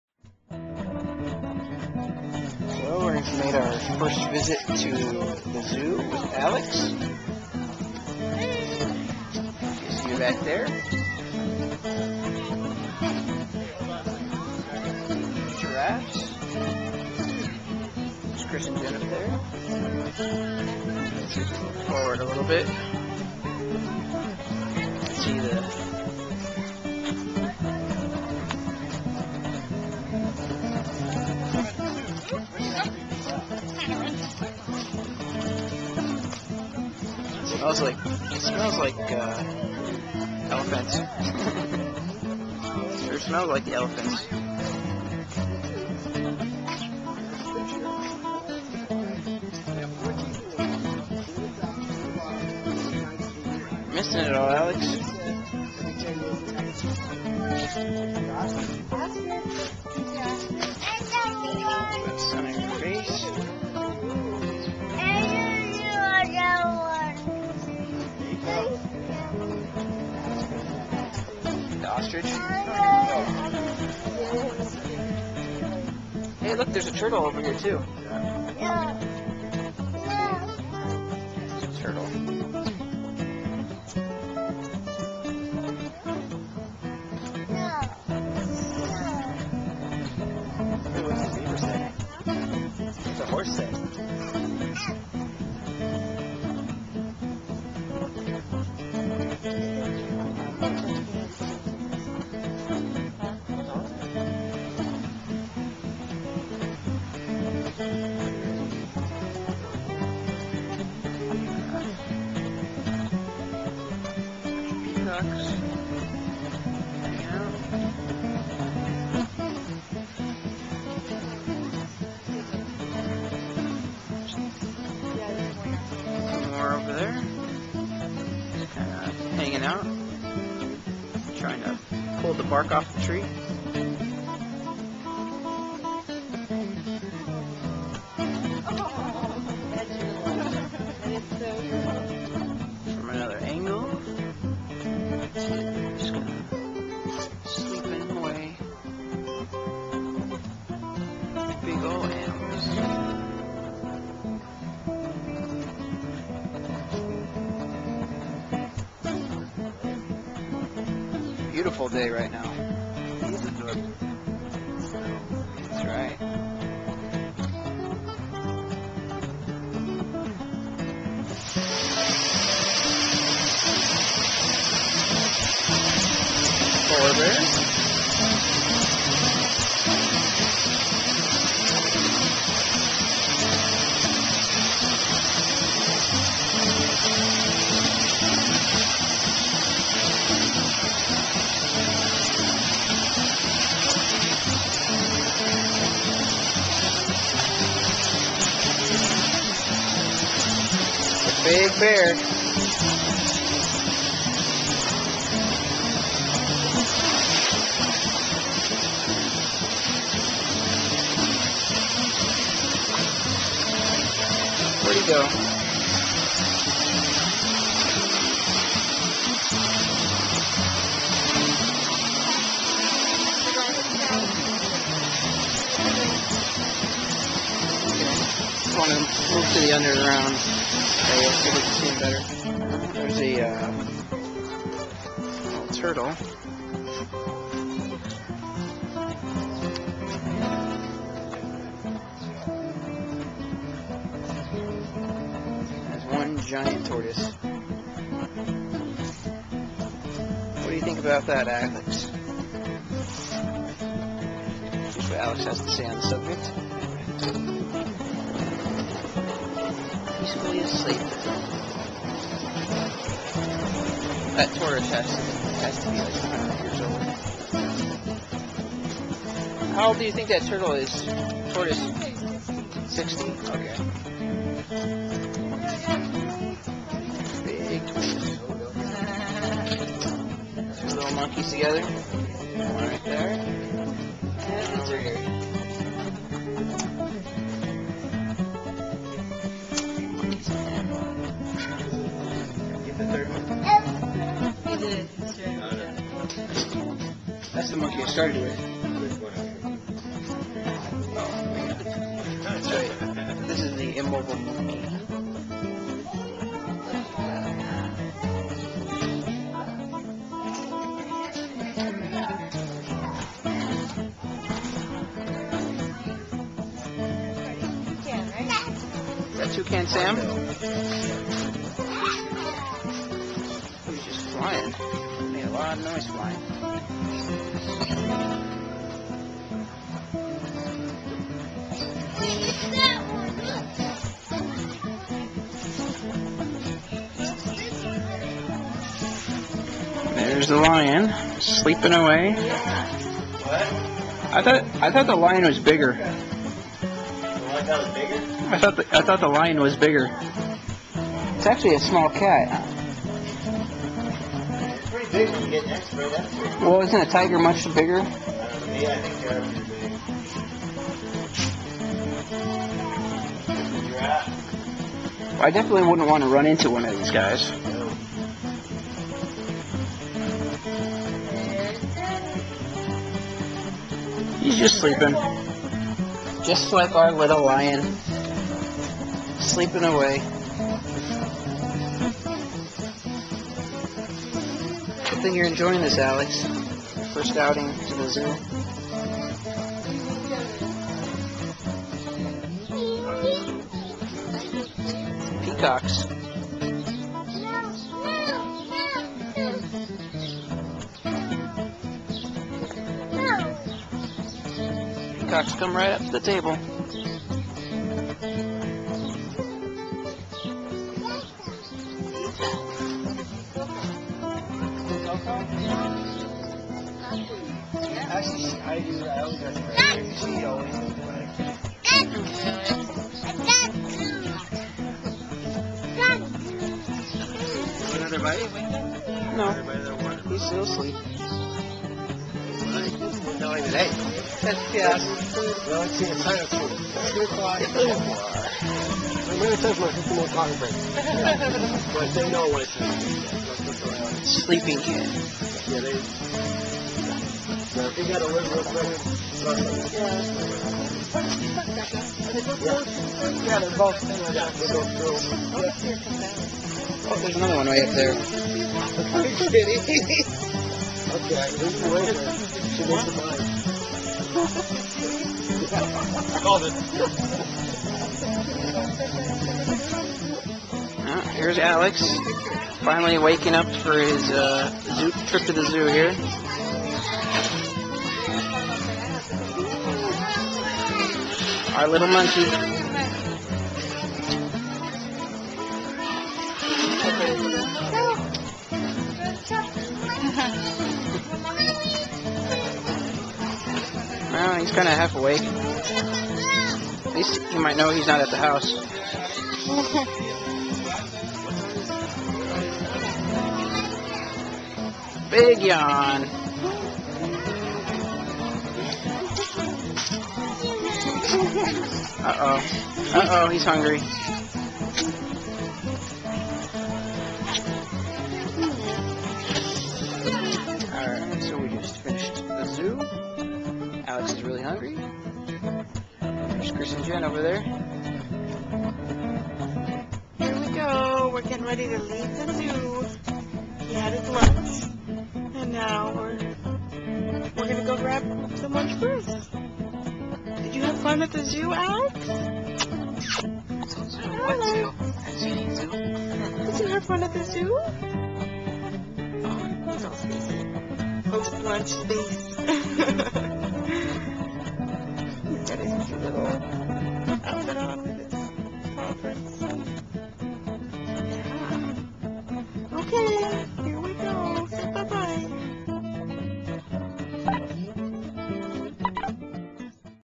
Alex's First Trip to the Reid Park Zoo
Created on November 1, 2009 using FlipShare. Alex goes to the Reid Park Zoo. Chris, Jen, and Fragetti join the fun.